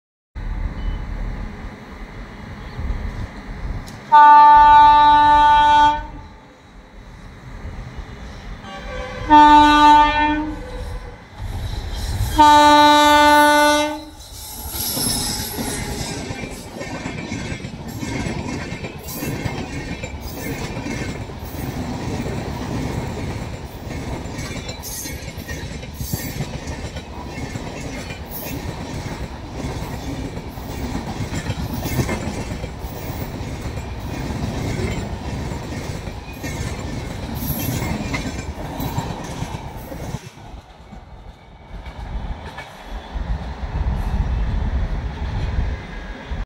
Chhattisgarh Exp zooms past Tughlakabad with remarkable acceleration showcasing its impressive speed
The 18237/18238 Chhattisgarh Express holds a prominent place as an iconic Indian train linking Gevra Road and Amritsar. Named after the state of Chhattisgarh, it traverses through Chhattisgarh, Maharashtra, Madhya Pradesh, Uttar Pradesh, Delhi, Haryana, and Punjab, covering an extensive distance of 2,109 kilometers.

Rail Travelers